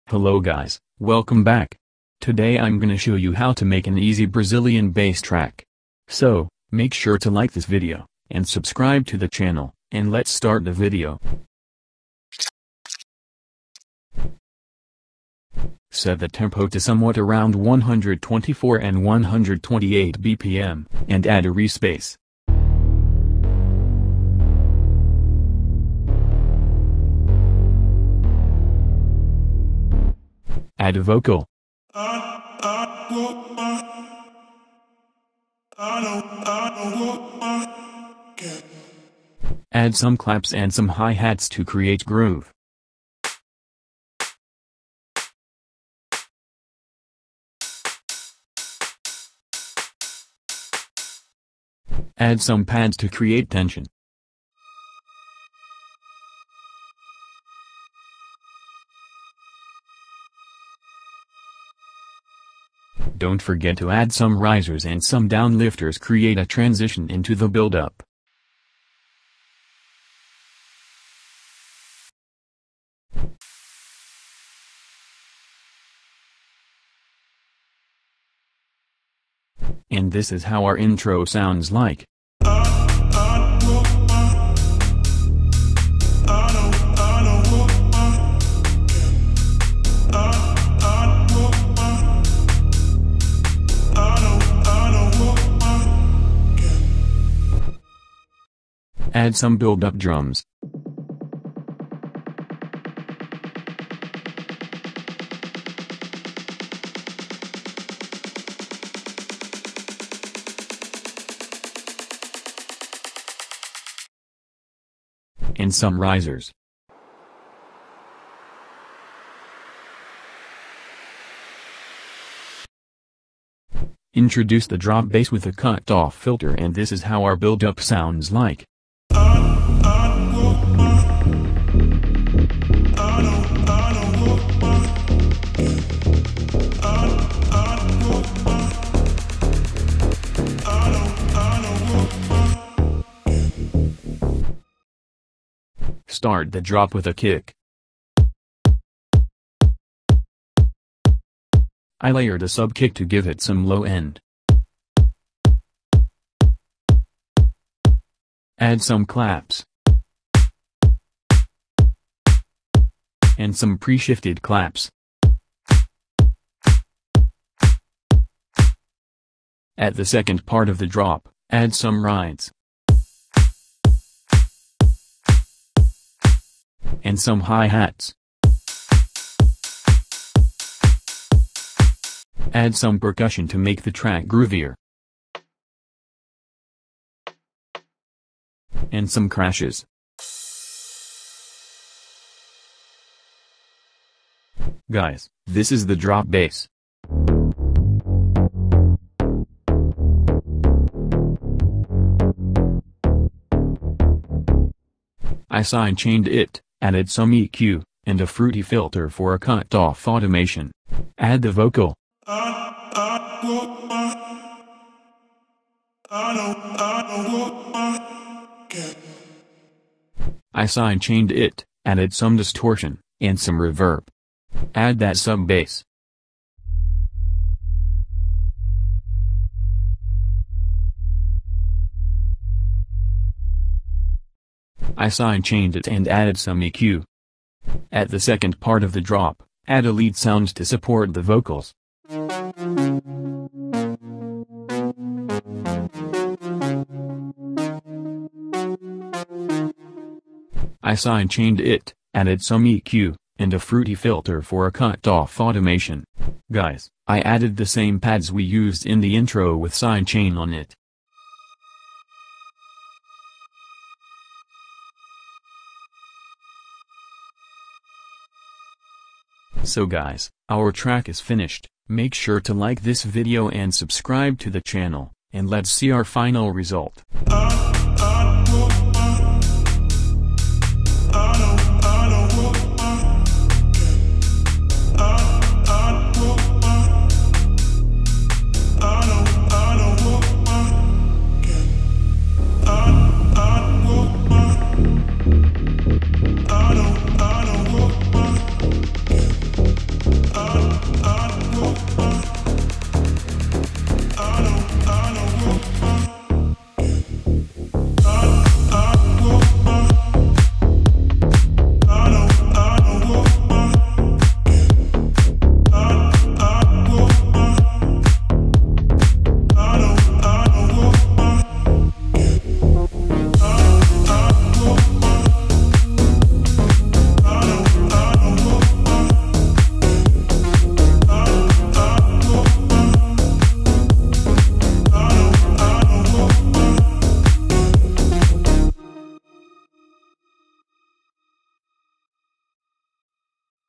How To Make Easy Brazilian Bass - FL Studio Tutorial (+FREE FLP)
"How To Make Easy Brazilian Bass - FL Studio Tutorial"
In this tutorial, I will show you how to make an easy Brazilian Bass  track.

Plugins used in this video:
Spire
Kickstart
CamelCrusher
FL Studio Stock Plugins